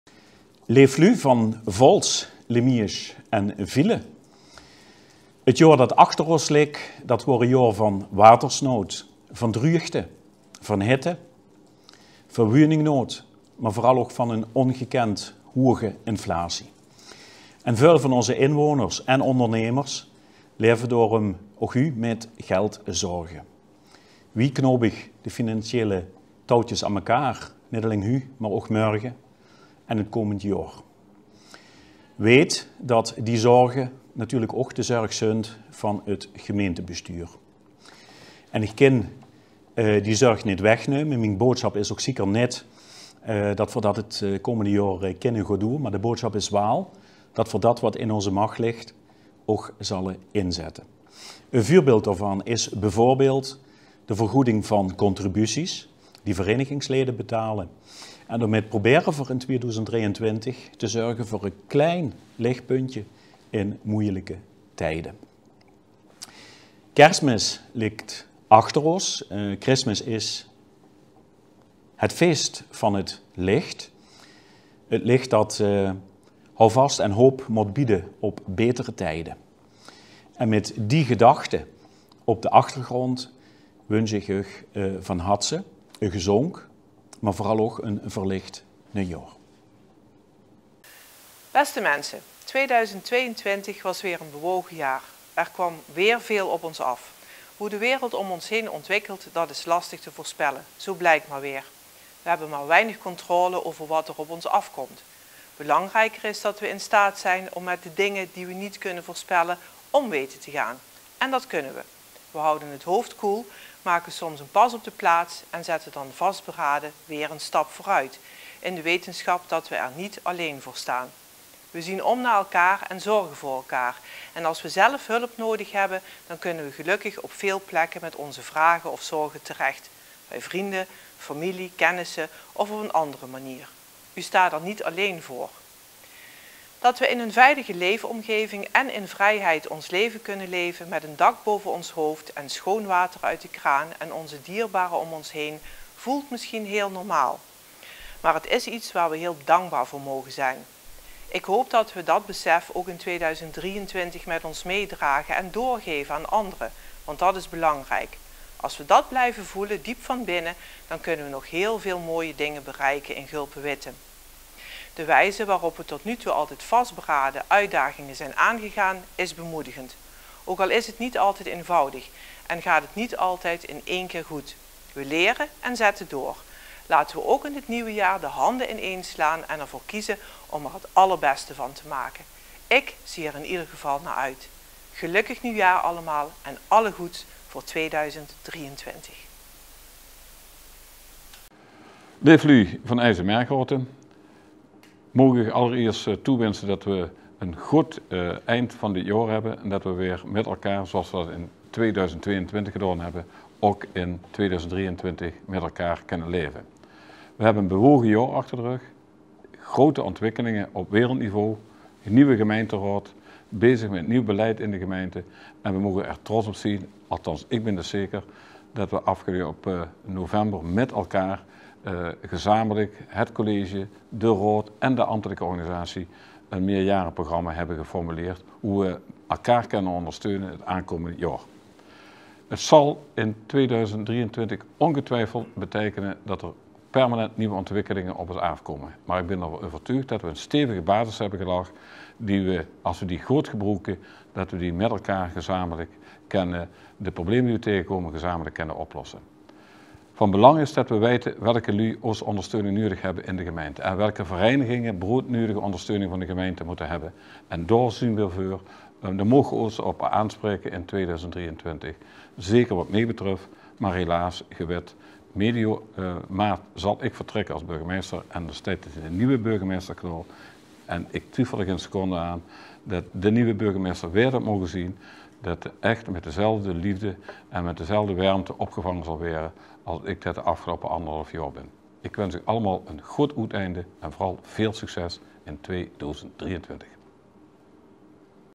Nieuwjaarswensen 2023 burgemeesters
De 3 burgemeesters van respectievelijk Vaals, Gulpen-Wittem en Eijsden-Margraten spreken hun wensen uit voor het nieuwe jaar.
Camera: Math Vanhautem, Mathieu Snijders